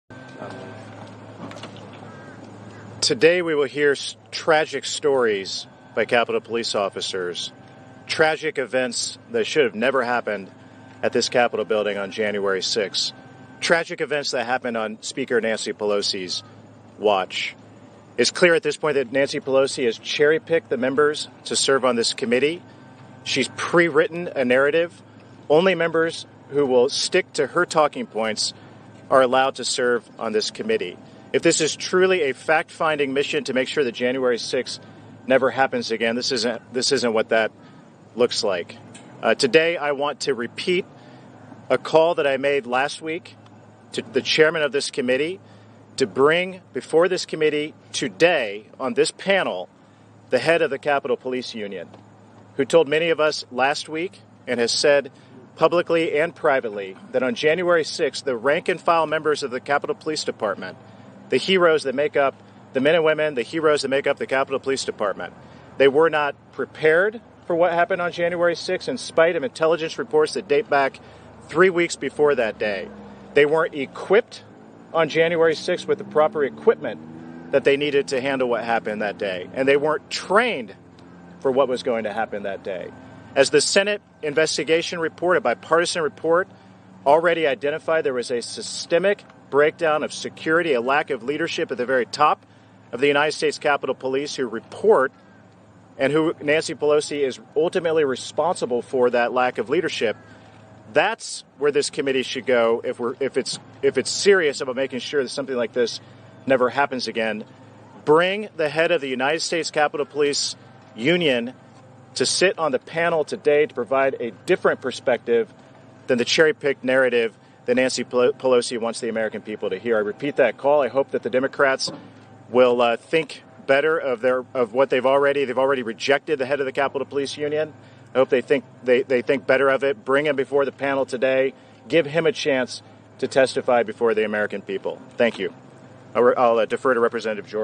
Rep. Banks: Pelosi Cherry-Picked The Members
"It's clear at this point that Nancy Pelosi has cherry-picked the members to serve on this committee. She has pre-written a narrative. Only members who will stick to her talking points are allowed to serve on this committee." - Rep. Jim Banks

About House Republicans:

Elise Stefanik (R-NY) is chair of the House Republican Conference.